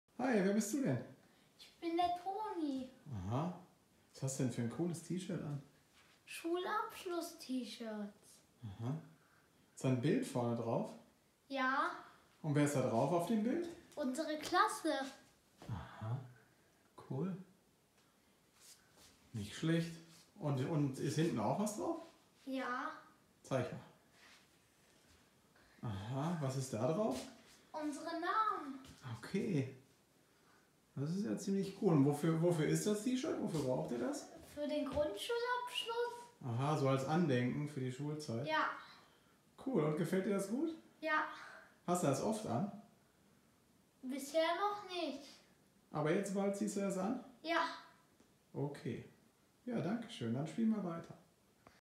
Grundschulshirt Tony
Stolz zeigt uns Tony sein neues Grundschulabschluss T-Shirt